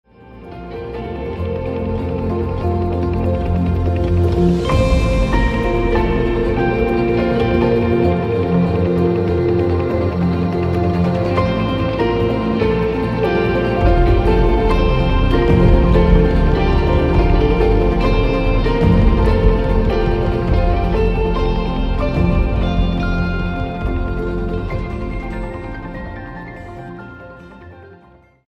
Honduras orgulloso de haber nacido allí en esa patria. Dennis Palacios
Avenida circunvalación en San Pedro Sula disfrútalo.